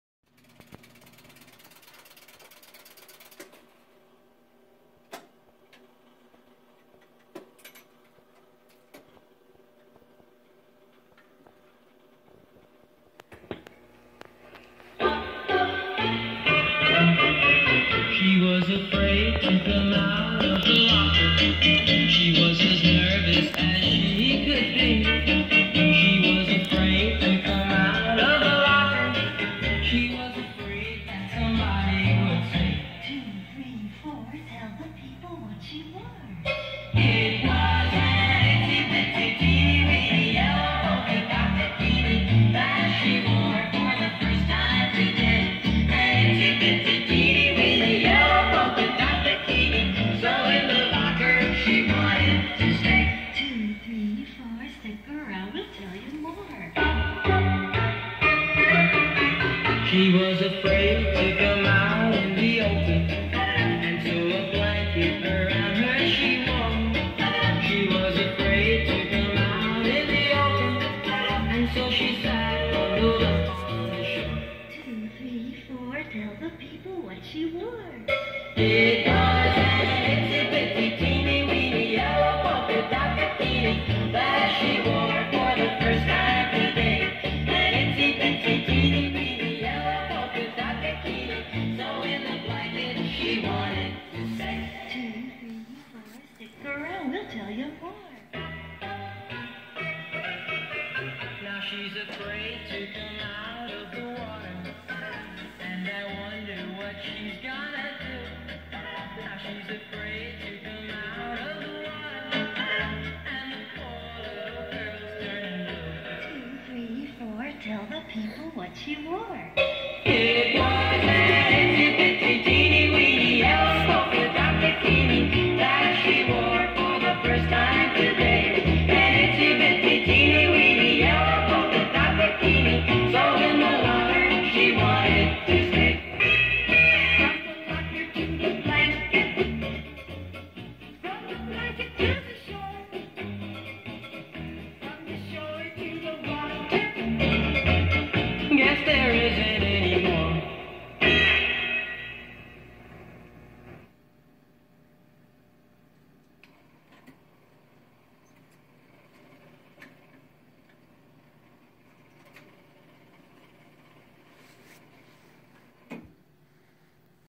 Wonderful Wurlitzer Model 2200 juke box sells at auction.
Wonderful Wurlitzer Model 2200 Multi-Selector Phonograph Juke Box. Serial No. 356186. This is full of period 45s and seems to work well. Come on baby, let's get the juke box!